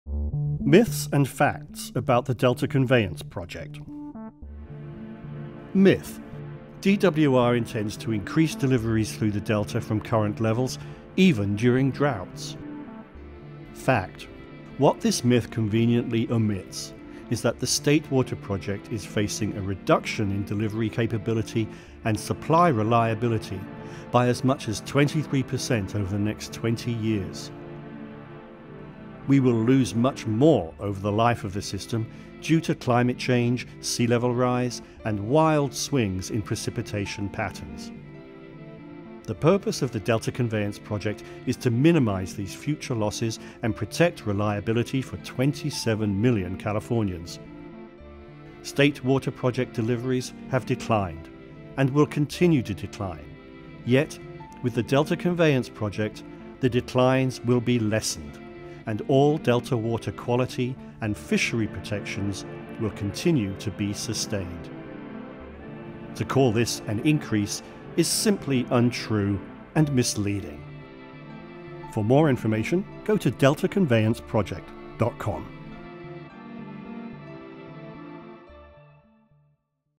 Myths & Facts About Delta Conveyance. Myth #14: DWR Will Increase Deliveries
Myth: DWR intends to increase deliveries through the Delta from current levels, even during droughts.

Fact: The Delta Conveyance Project (DCP) will minimize future water supplies losses due to climate change.  With DCP the declines will be lessened while all Delta water quality and fishery protections will continue to be sustained.

Check out the Delta Conveyance Project Myths and Facts for corrections to common misconceptions about this critical climate adaptation project.